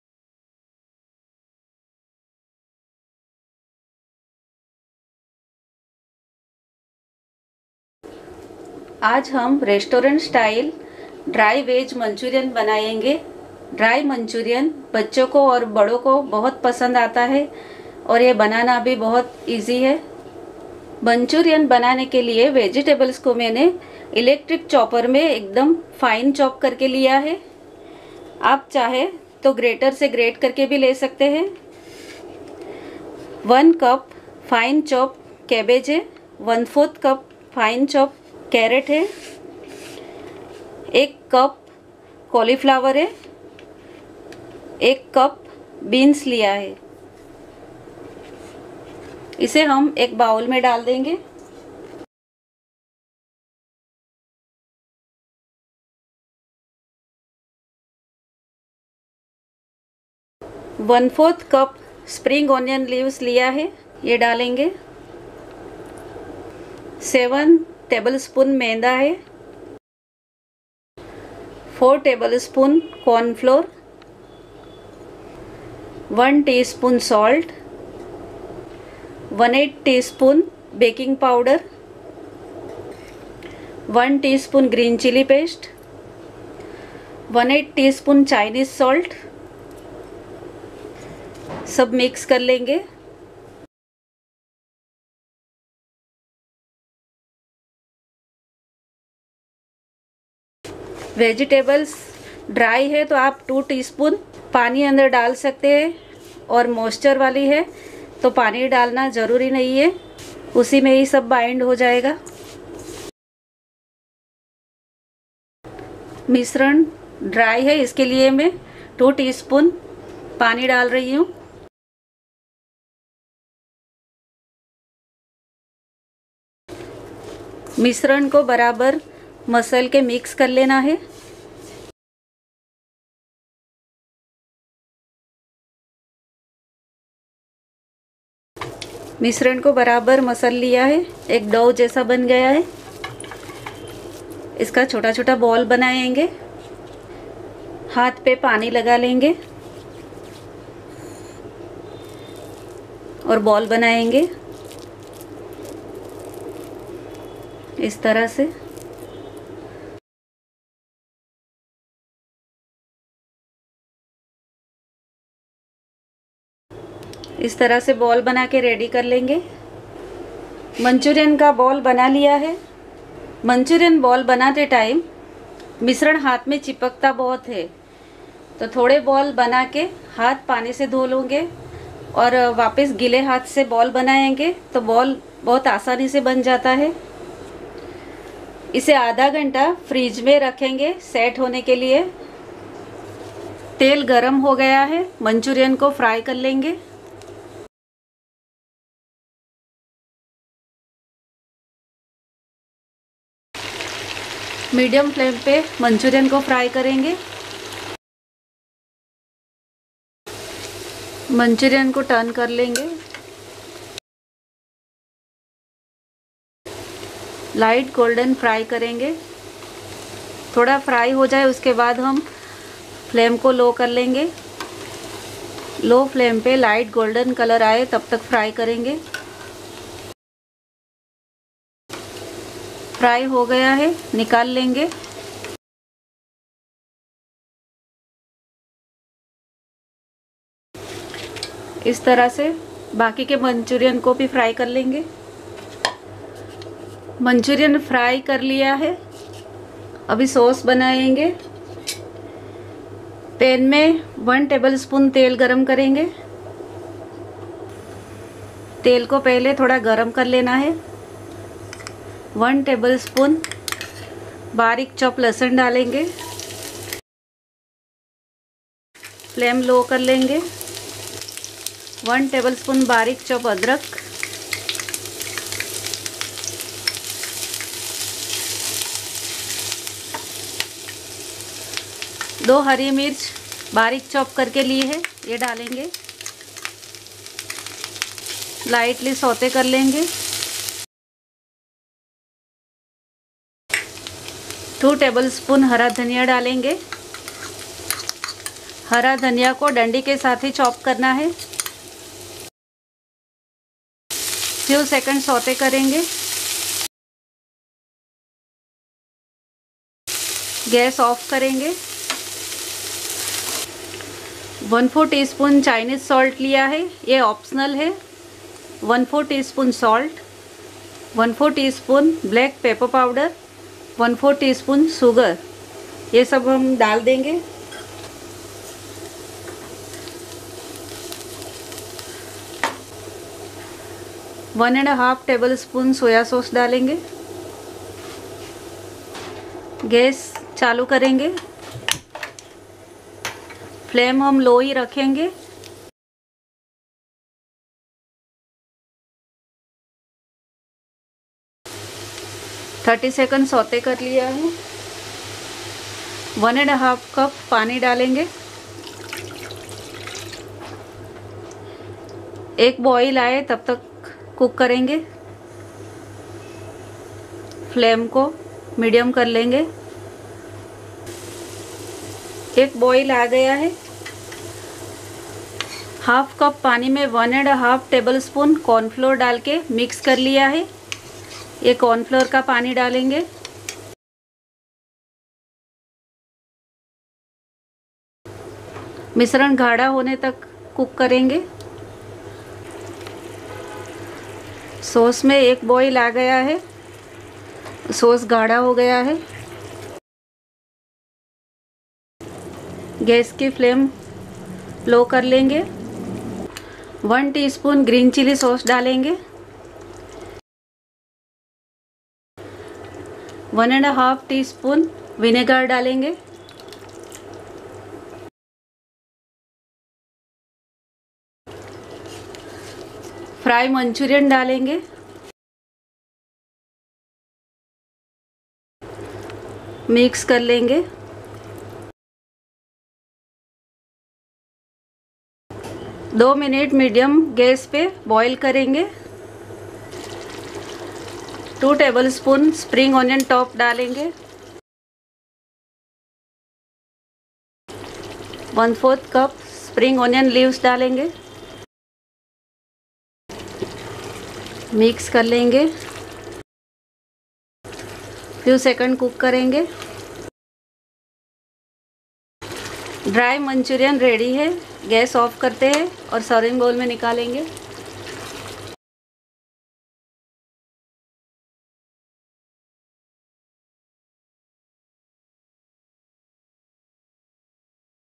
Veg Manchurian Dry Starter - How to make perfect Manchurian Balls - Dry Manchurian Recipe in Hindi
Tasty Veg Manchurian dry starter recipe, this amazing classic manchurian appetizer video teaches how to make perfect manchurian balls, do try this tasty manchurian indochinese recipe at home.

Vegetable Manchurian Dry Recipe Ingredients

1 cup Patta Gobi finely chopped (Cabbage)
1/4 cup Gajar finely chopped (Carrot)
1 cup Gobi finely chopped (Cauliflower)
1 cup Fansi finely chopped (Green Beans)
1/4 cup Hara Pyaz finely chopped (Spring onion)
7 Tbl Sp Maida(Refined flour)
4 Tbl sp Corn Flour (Finely Ground Maize)
1 Tsp salt
1/8 Tsp Baking powder
1/8 Tsp Hari Mirch ki Paste (Green chilli paste)
1/8 Tsp Chinese salt
2 Tsp water
1 Tbl sp oil
1 Tbl sp Lasun finely chopped (Garlic)
1 Tbl sp Adrak finely chopped(Ginger)
2 Hari Mirch finely chopped (Green chilli)
2 Tbl sp Hara Dhania Dandi ke sath finely chopped (Green coriander leaves with stem finely chopped)
1/4 Tsp Chinese salt (Optional)
1/4 Tsp salt
1/4 Tsp Kali Mirch powder (Black pepper powder)
1/4 Tsp Chini (Sugar)
1 1/2 Tbl sp Soya sauce
1 1/2 cup water
1/2 cup water+ 1 1/2 Tbl sp Corn Flour (Finely Ground Maize)
1 Tsp Green Chilli sauce
1 1/2 Tsp Vinegar
2 Tbl sp Hara Pyaz white part finely chopped (Spring onion white part)
1/4 cup Hara Pyaz ki Patti finely chopped (Green onion leaves finely chopped)

Enjoy this amazing veg manchurian dry starter with your friends and family.